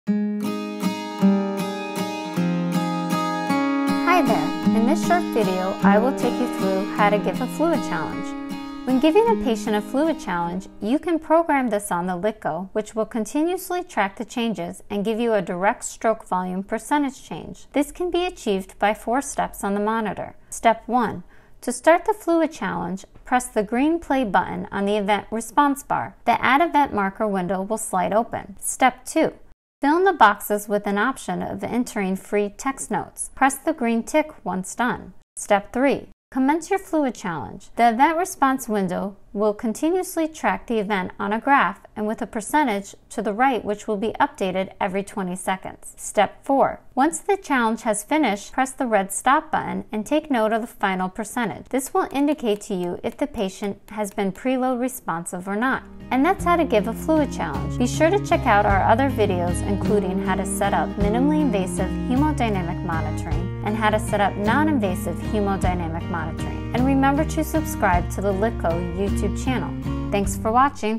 How to give a fluid challenge using LiDCO hemodynamic monitor
This is a tutorial video showing how to setup the LiDCO hemodynamic monitor for minimally invasive hemodynamic monitoring.